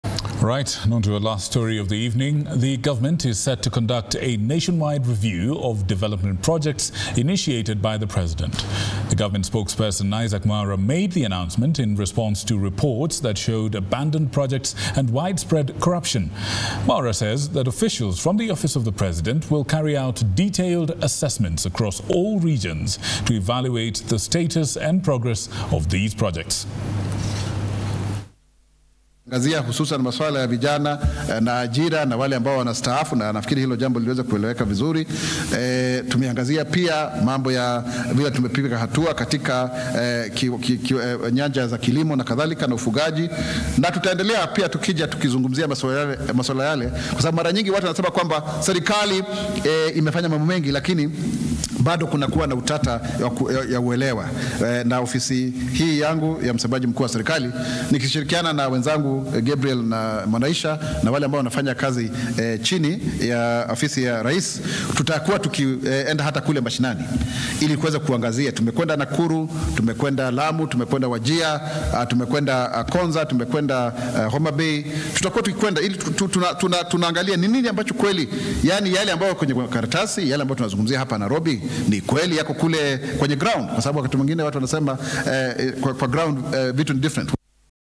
Government set to conduct nationwide review of development projects
The government is set to conduct a nationwide review of development projects initiated by the president. Government Spokesperson Isaac Mwaura made the announcement in response to reports that showed abandoned projects and widespread corruption.